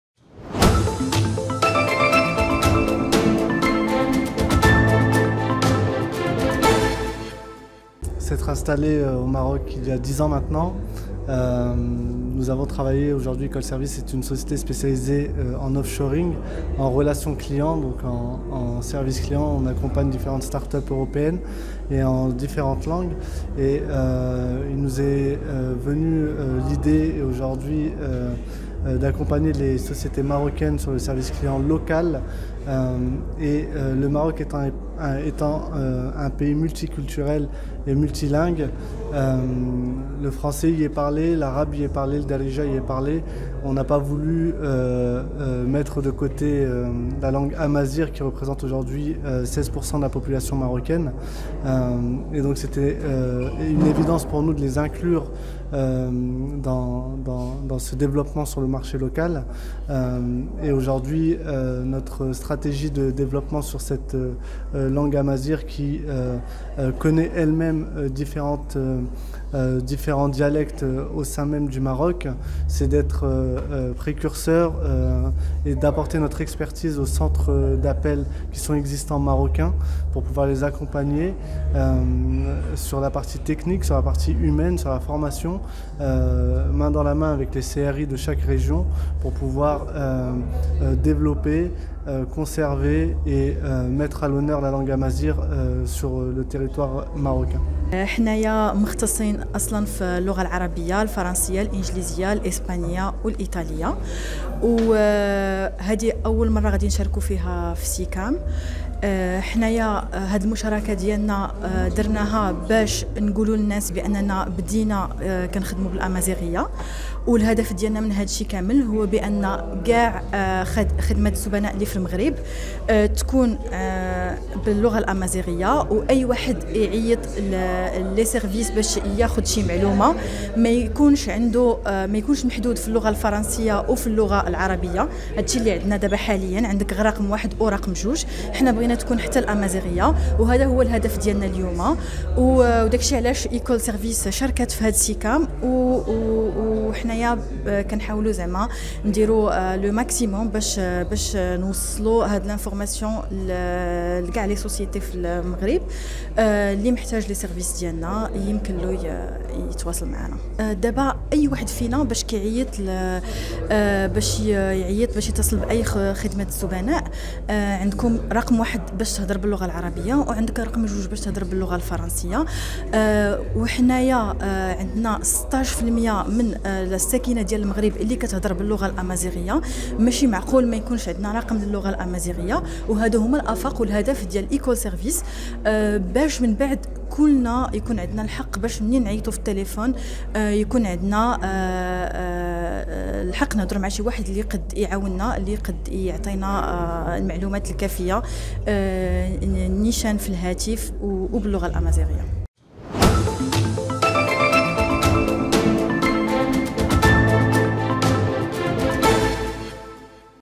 تعرف على أول مركز اتصال يستعمل الأمازيغية بالمغرب
في سابقة من نوعها بالمغرب، تم الكشف عن اول مركز اتصال  E-Call Services يستعمل الأمازيغية وذلك خلال المعرض الدولي لمراكز خدمات الاتصال- سيكام Siccam، الذي نظم  من 20 إلى 21 يونيو 2023 في الدار البيضاء، تحت شعار: "الذكاء الاصطناعي: مخاطر وفرص قطاع الخدمات المرحلة  بالمغرب".
وذلك بمشاركة حوالي ثلاثين عارضًا في المجال الرقمي، والخدمات المرحلة، وإدارة علاقات العملاء في المغرب، يمثلون ستة بلدان من إفريقيا وأوروبا: بلجيكا وسويسرا ومصر وموريتانيا وفرنسا، بالإضافة إلى السلفادور ، التي ستستضيف فعاليات المعرض للسنة المقبلة 2024.